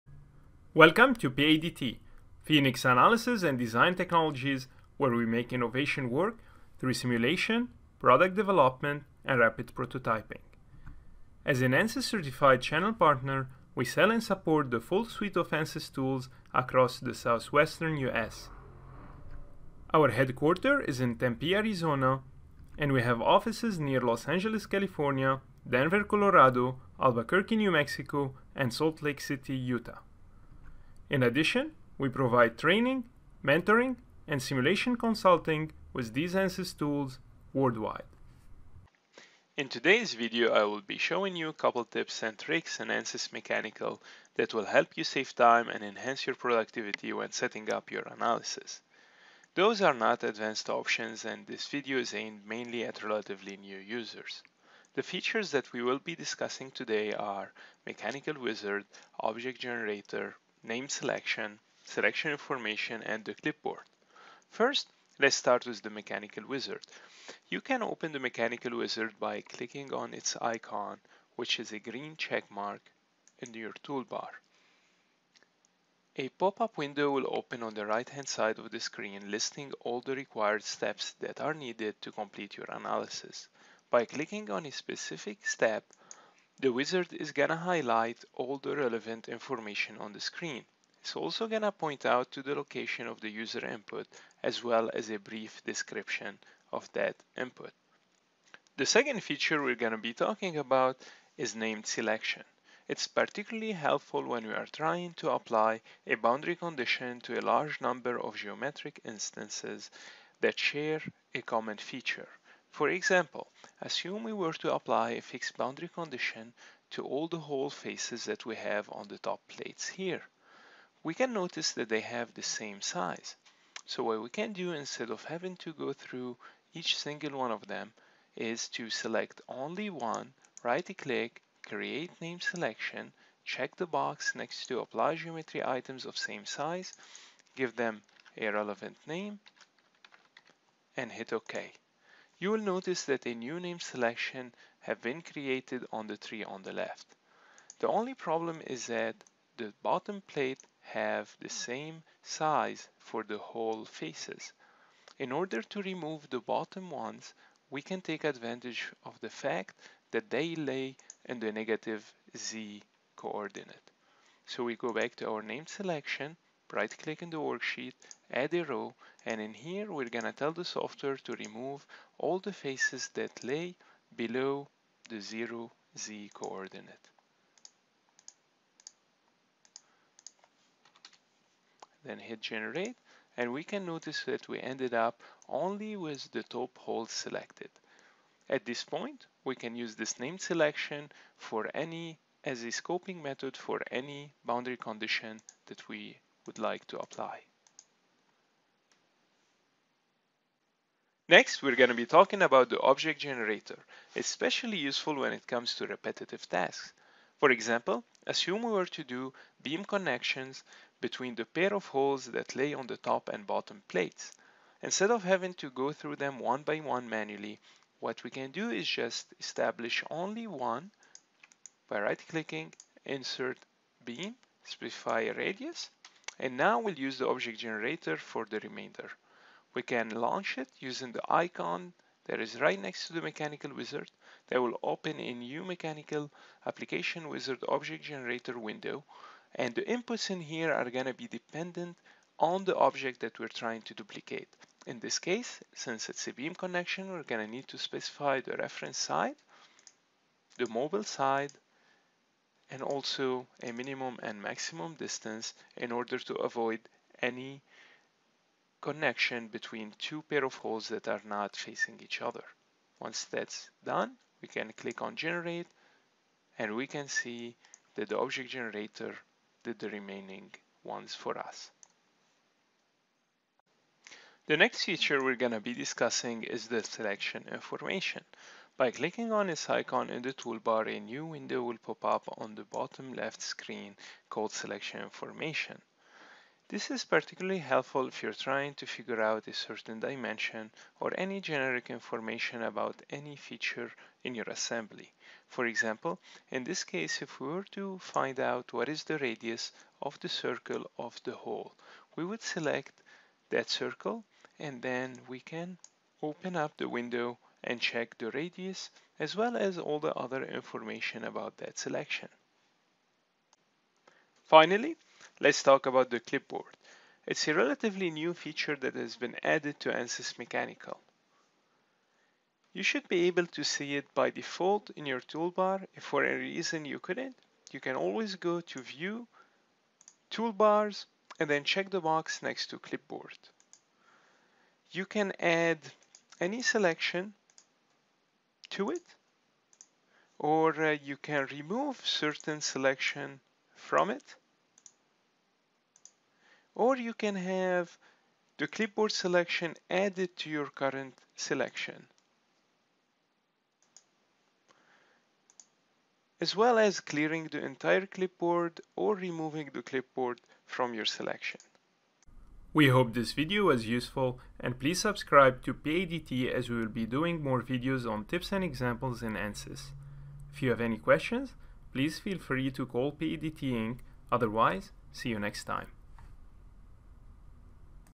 Ziad's ANSYS Mechanical Tips and Tricks
PADT support engineer, Ziad Melhem, shares a collection of useful tips and tricks that every user of ANSYS Mechanical should know. These shortcuts will let all users, new or experienced, be more productive when doing their simulations.